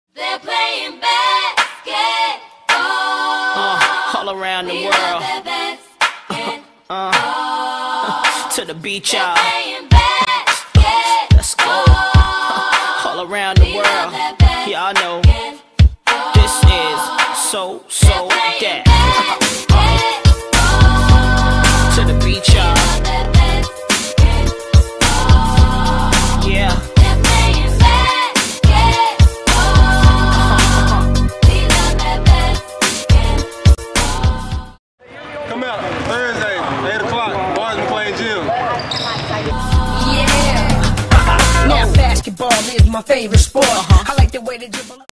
present SigmaWeek2k15 SIGMA TV: THURSDAY
Your Baylor Sigmas take on the Baylor Alphas in basketball at 8 P.M. Thursday March 26, 2015 at the Marrs Mclean Gym on campus. Come out and support the entertainment for Sigma Week!